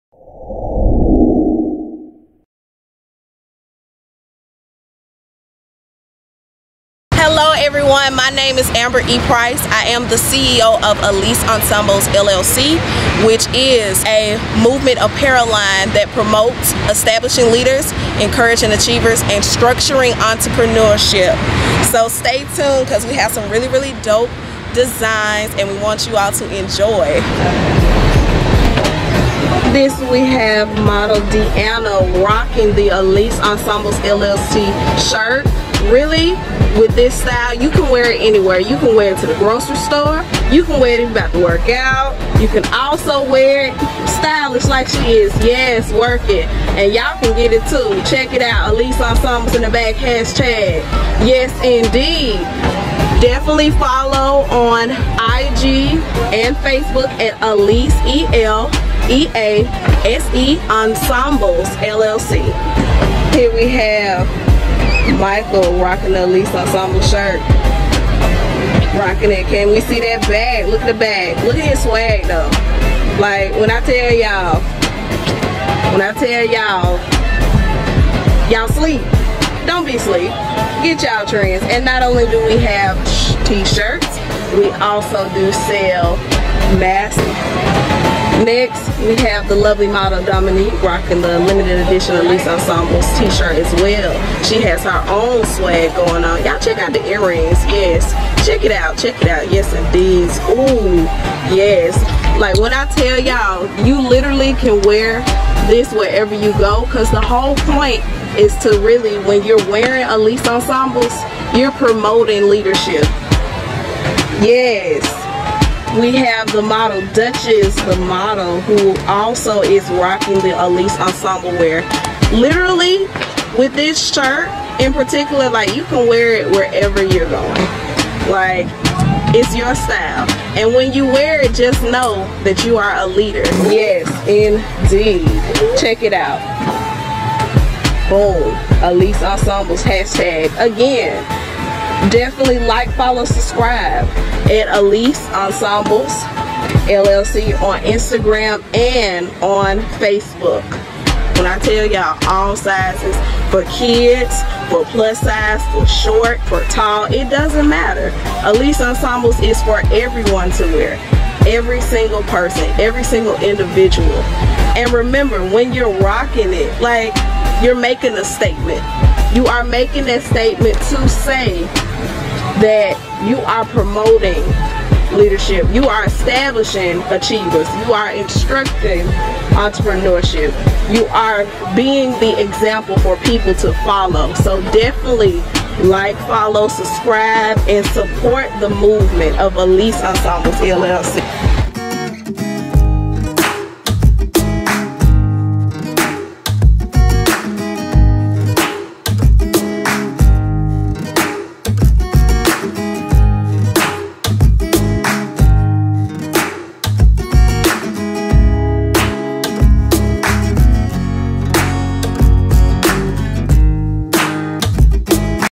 OH SNAP ENSEMBLES!:E.L.E.A.S.E. Ensembles LLC Vlog
ENSEMBLES! Watch the Video to Find Out More!

What is ELEASE ENSEMBLES LLC?
E.L.E.A.S.E. Ensembles L.L.C. is a movement apparel company founded in February 2020 that promotes Establishing Leaders, Encouraging Achievers, and Structuring Entrepreneurship. The company was named after the C.E.O. Amber E. Price, native from Birmingham, AL middle name. It is derived from my grandmother’s first name and mother’s middle name. This helped inspire me to create her company and emphasize the importance of becoming your boss and accomplishing whatever goals that you would want to achieve while being an influential person in everyone that you come across in your everyday walk.